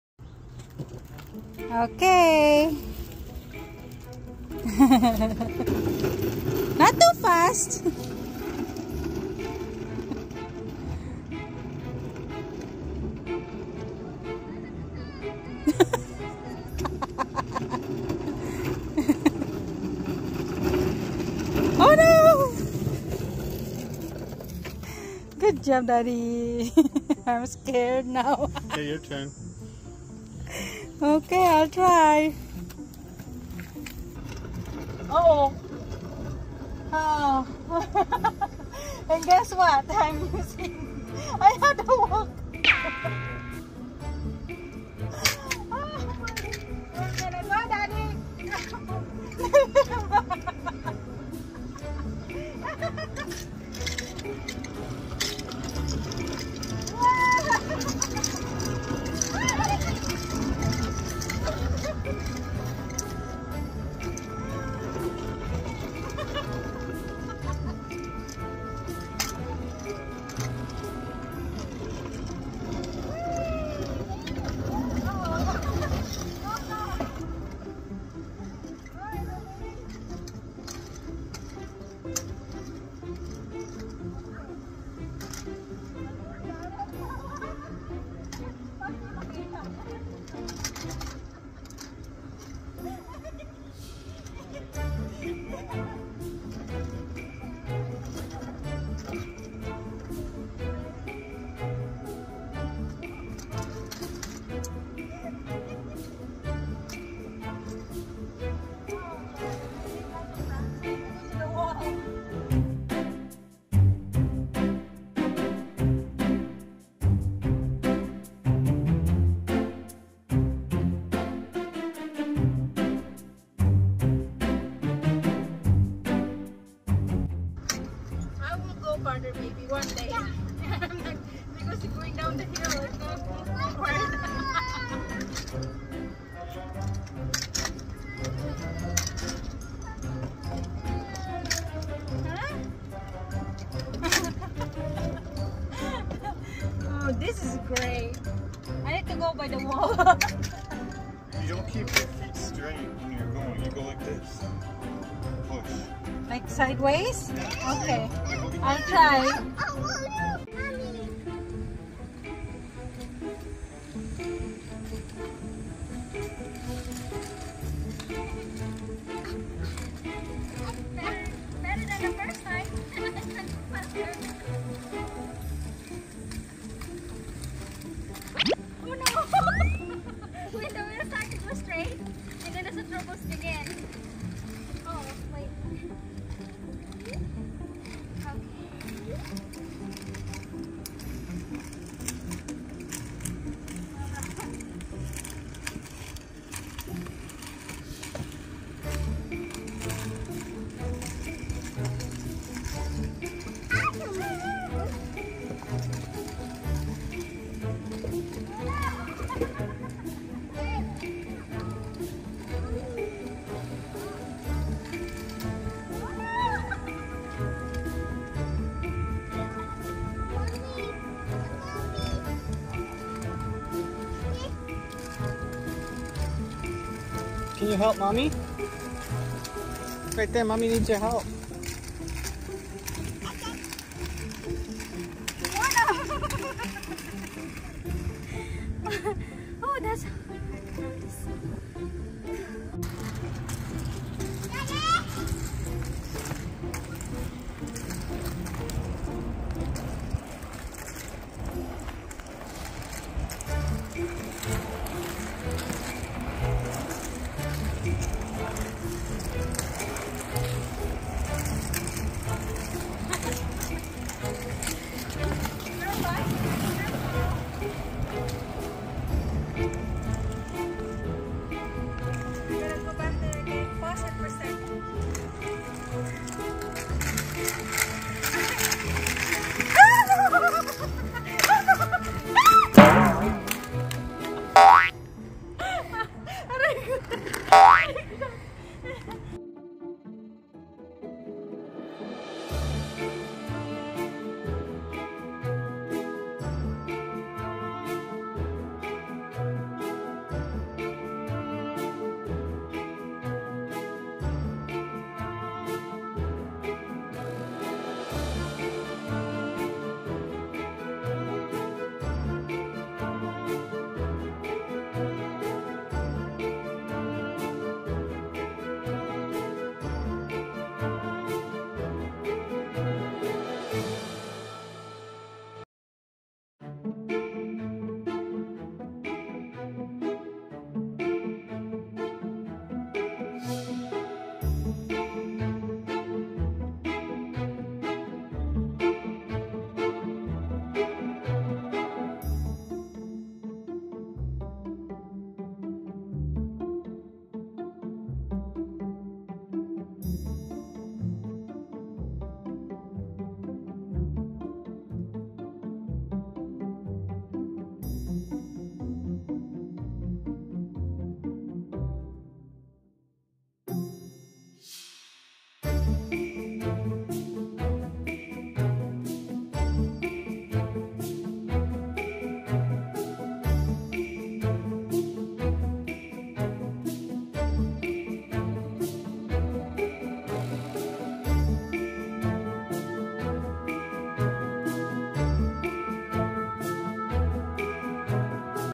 Practicing Roller Blading | Family Time 💚
I purchased the Walker from Thrift Store and planning to send it home (Philippines) but unfortunately, it doesnt fit in the box. few days after we decided to have fun with roller blading and I remember to try using the walker instead. 😆 it works but still i lost my balance. 😆. Baby MK had so much as well specially when daddy was pushing her at the same time🥰🥳